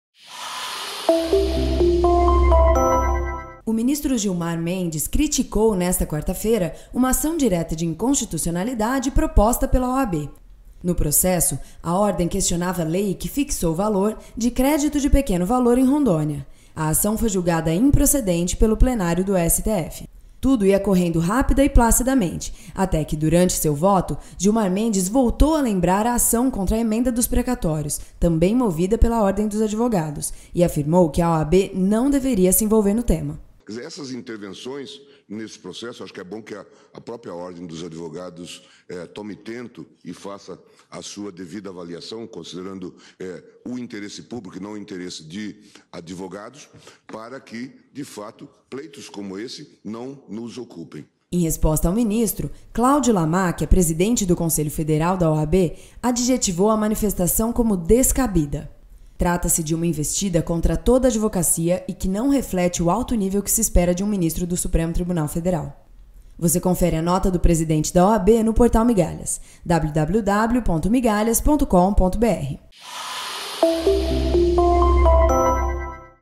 Gilmar X Lamachia
O presidente do Conselho Federal da OAB, Cláudio Lamachia, divulgou nota acerca de “descabida manifestação” proferida pelo ministro do STF Gilmar Mendes na sessão da manhã desta quarta-feira, 7 de fevereiro, durante julgamento da ADIn 4.332.